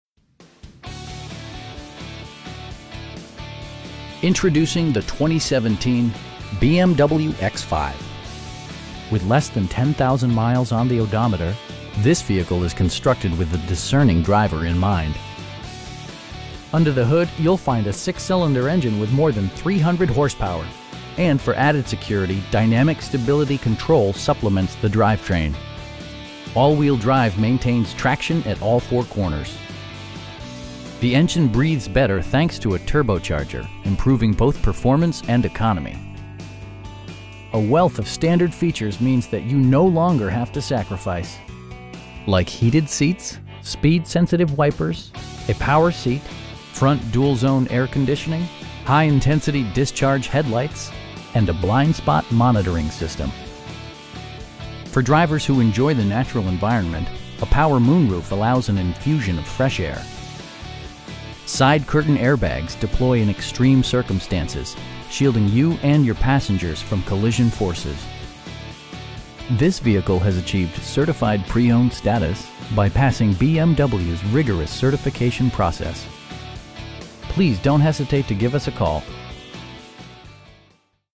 2017 BMW X5 xDrive35i in Creve Coeur, MO 63141
Plaza BMW
11858 Olive Blvd

Introducing the 2017 BMW X5 With less than 10,000 miles on the odometer, this vehicle is constructed with the discerning driver in mind. Under the hood you'll find a 6 cylinder engine with more than 300 horsepower, and for added security, dynamic Stability Control supplements the drivetrain. All wheel drive maintains traction at all four corners. The engine breathes better thanks to a turbocharger, improving both performance and economy. A wealth of standard features means that you no longer have to sacrifice. Like heated seats, front and rear reading lights, speed sensitive wipers, a power seat, front dual-zone air conditioning, high intensity discharge headlights, lane departure warning, and a blind spot monitoring system. For drivers who enjoy the natural environment, a power moon roof allows an infusion of fresh air. Side curtain airbags deploy in extreme circumstances, shielding you and your passengers from collision forces. This vehicle has achieved Certified Pre-Owned status, by passing BMW's rigorous certification process. We pride ourselves in the quality that we offer on all of our vehicles. Please don't hesitate to give us a call.